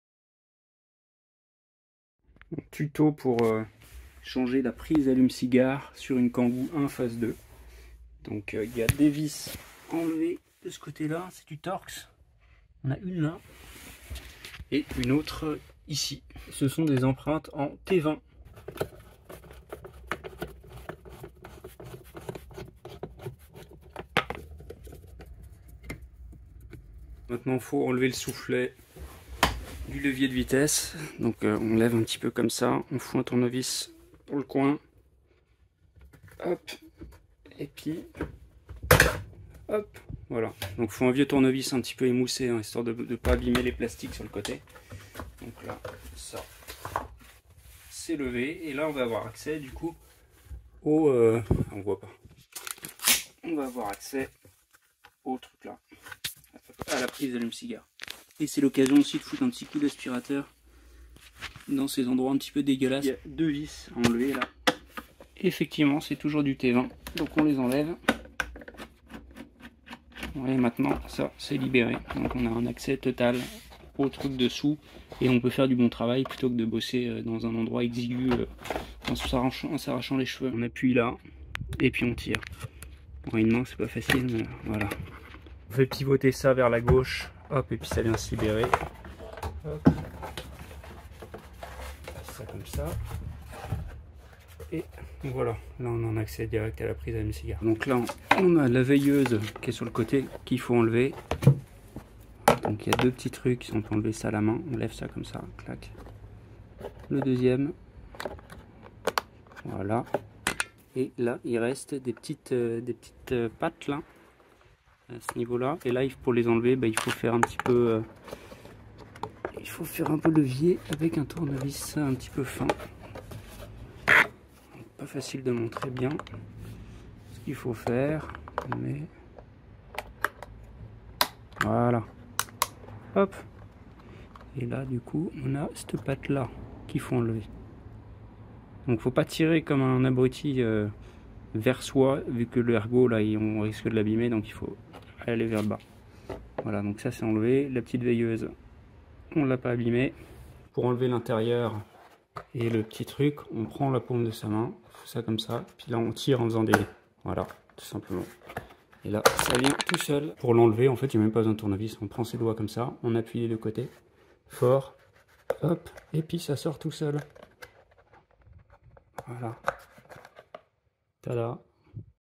Démonter prise allume cigare Kangoo 1 phase 2
Voici comment démonter proprement une prise allume cigare sur un Renault Kangoo 1 phase 2, le mien est de 2003, essence 1.2l 16v